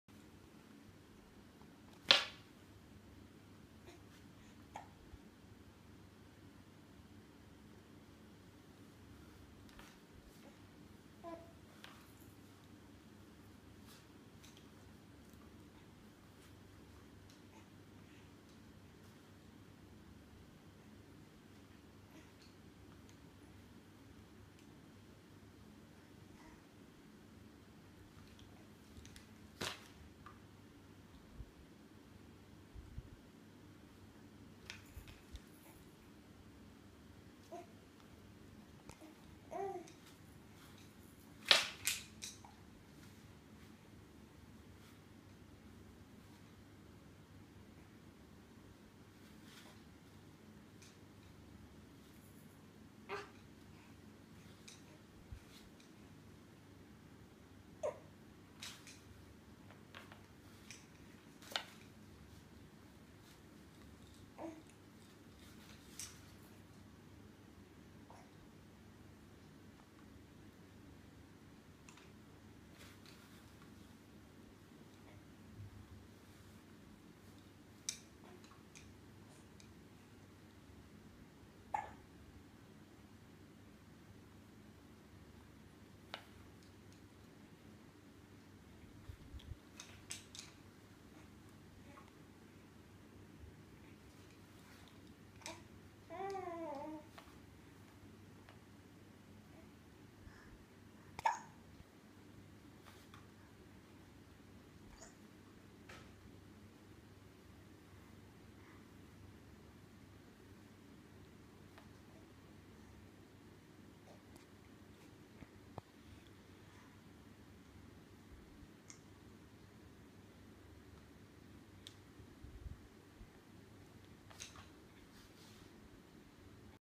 Blake playing on his mat
I want to remember how he plays on the mat :)
5 months old
10-4-13